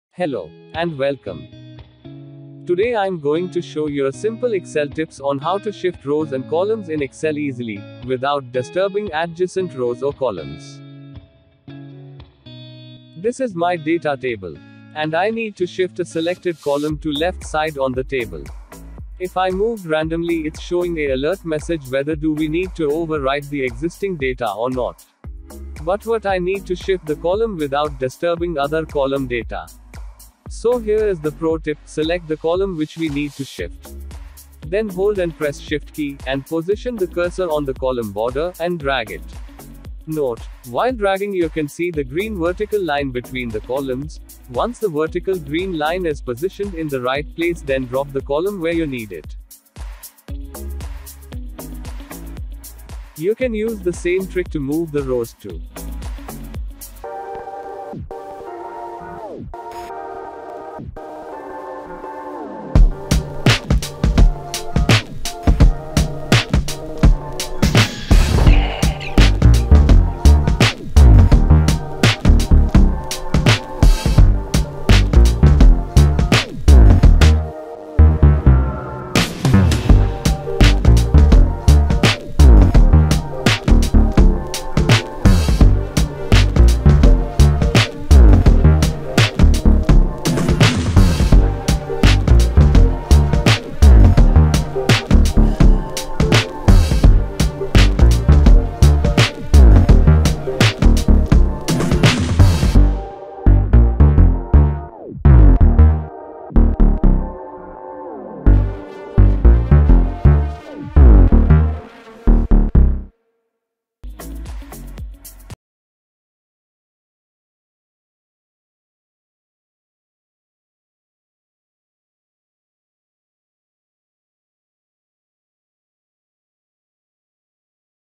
Excel Tips & Tricks: How to move Rows & Column (The Easier way!!)
Hello Friends! In this video, I have shown you a simple excel trick on how to shift or move the columns and rows in Excel.